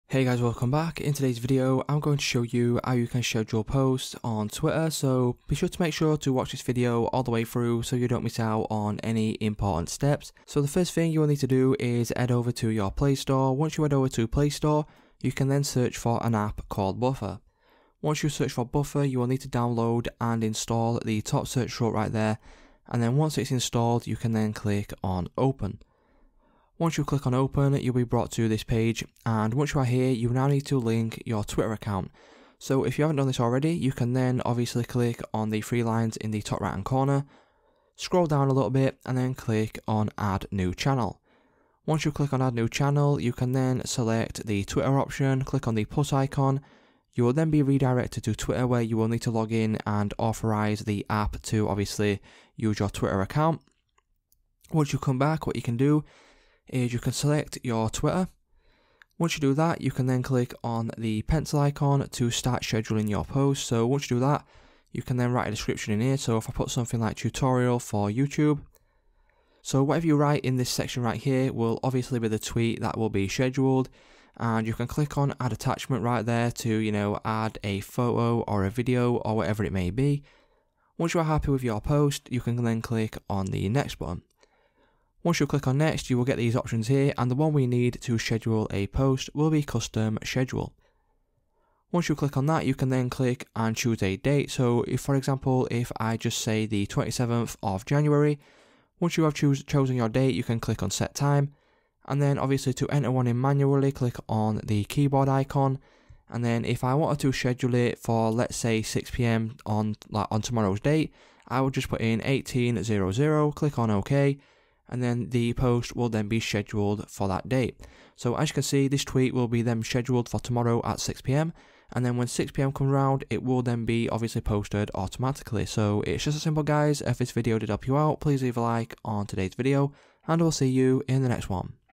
How to Schedule Posts on Twitter (2023)
Today I show you how you can schedule posts on your Twitter account.